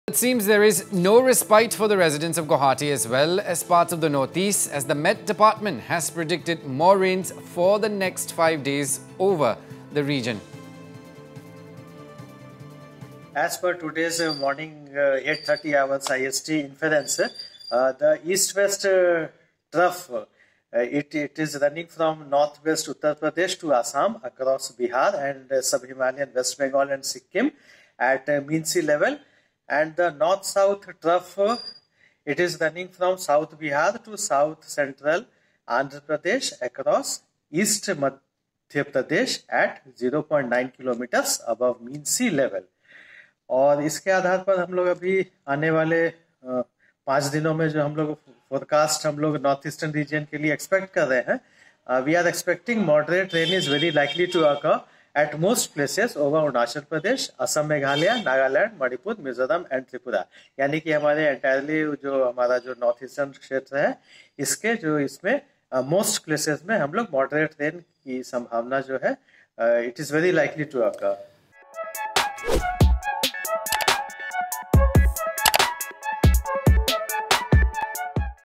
More rainfall predicted in NE for next 5 days
𝐌𝐨𝐫𝐞 𝐫𝐚𝐢𝐧𝐟𝐚𝐥𝐥 𝐩𝐫𝐞𝐝𝐢𝐜𝐭𝐞𝐝 𝐢𝐧 𝐍𝐄 𝐟𝐨𝐫 𝐧𝐞𝐱𝐭 𝟓 𝐝𝐚𝐲𝐬 | It seems there is no respite for the residents of Guwahati as well as parts of Northeast as the Meteorological department has predicted more rains for the next 5 days over the region.